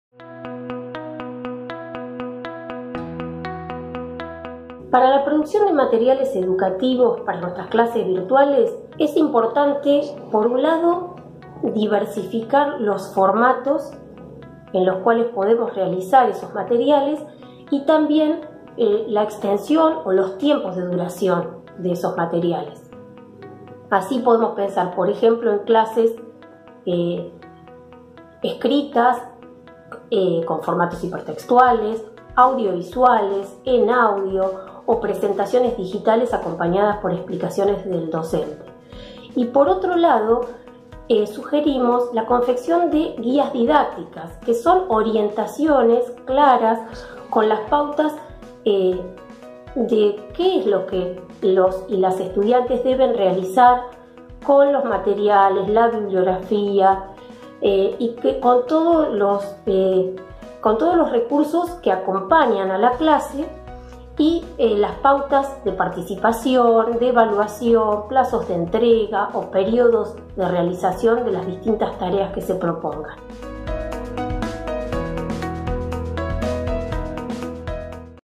Aportes para repensar los aprendizajes en contextos de educación en emergencia - Micro 6
Serie de recomendaciones útiles al momento de pensar un material educativo y que aconsejan tener en cuenta la diversificación y extensión de los formatos; así como diversos recursos que permiten enriquecer su producción, al igual que la combinación de clases escritas con formatos hipertextuales o audiovisuales, con presentaciones digitales acompañadas por explicaciones de los y las docentes.